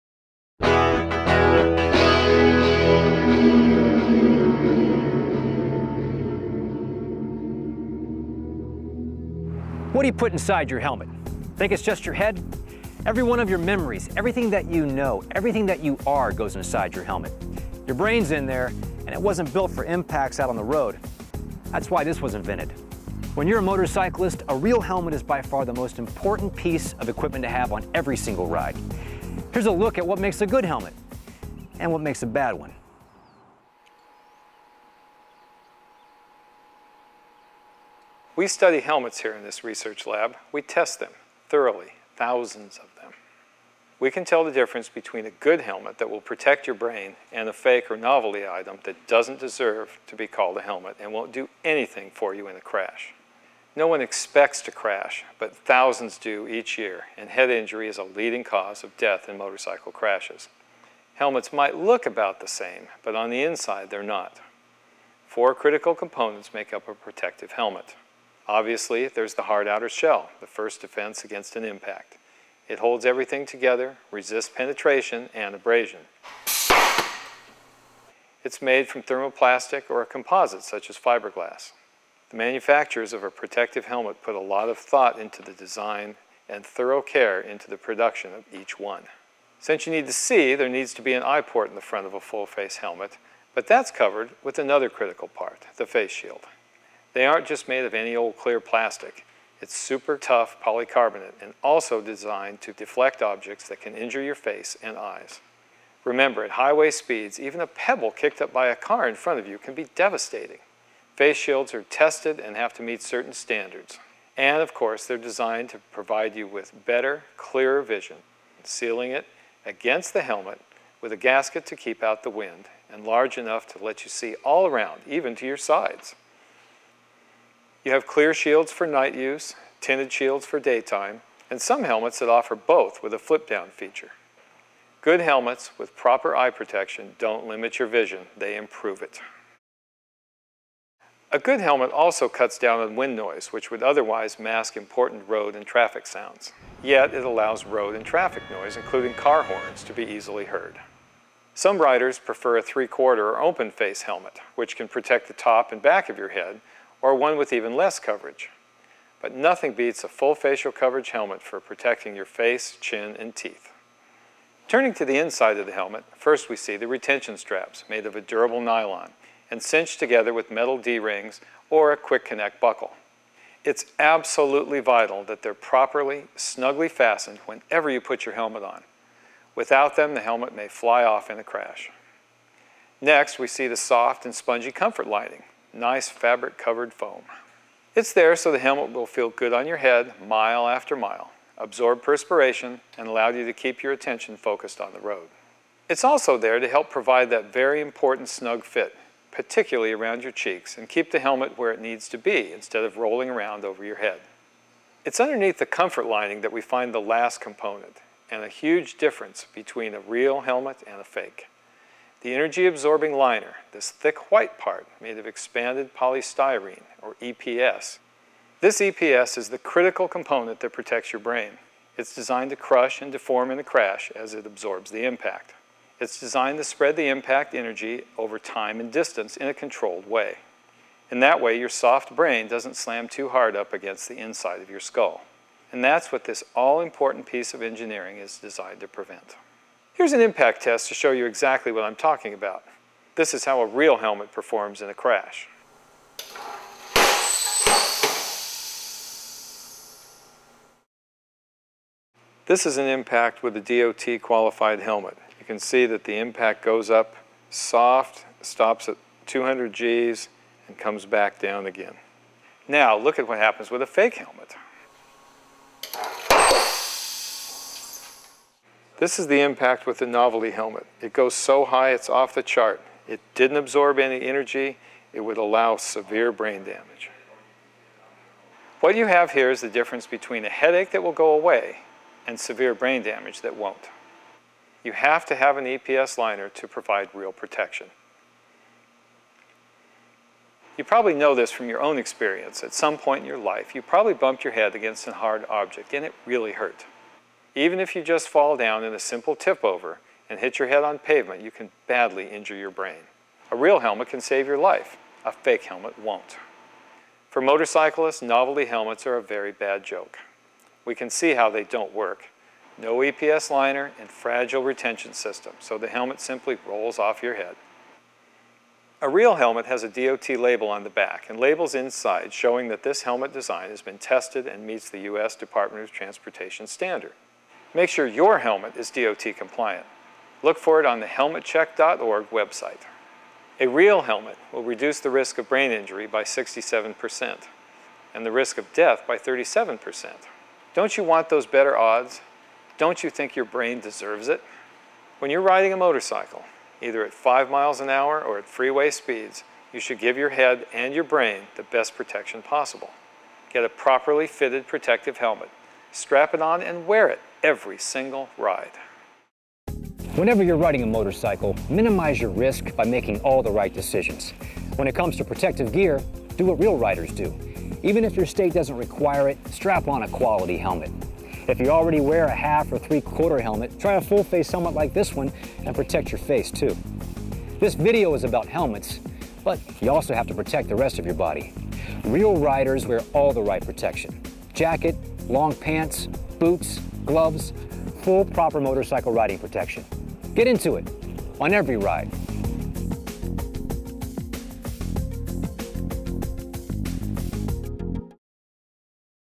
The Importance of Real Helmets
Produced by the Motorcycle Industry Council, this 6-minute video delves into the science behind motorcycle helmets, with special emphasis on the real-world differences between DOT-compliant and novelty helmets. A highlight of the video is a fascinating slow-motion scene of a helmet impact test (filmed at 1,000 frames per second) conducted at Collision and Injury Dynamics, Inc., showing how a quality helmet absorbs and dissipates crash forces.